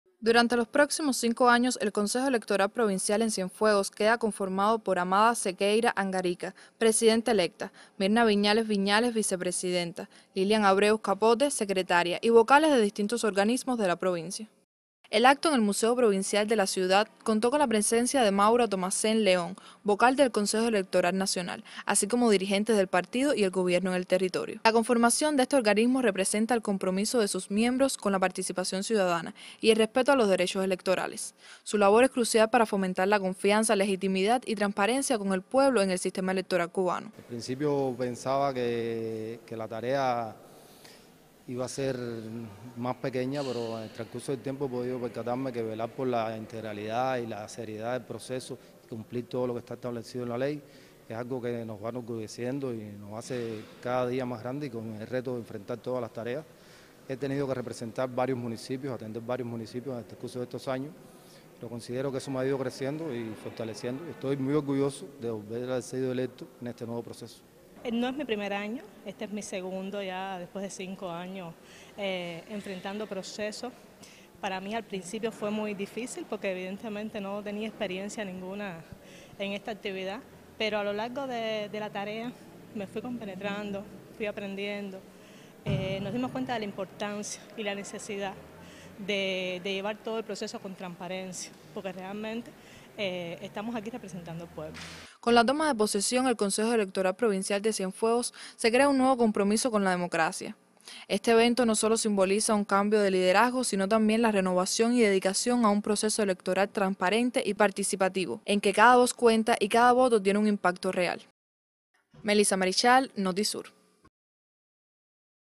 Tomó posesión Consejo Electoral Provincial de Cienfuegos
El Consejo Electoral Provincial de Cienfuegos tomó posesión para conducir procesos en el territorio. Este órgano del Estado tiene como misión fundamental organizar, dirigir y supervisar las elecciones, consultas populares, plebiscitos y referendos que se convoquen.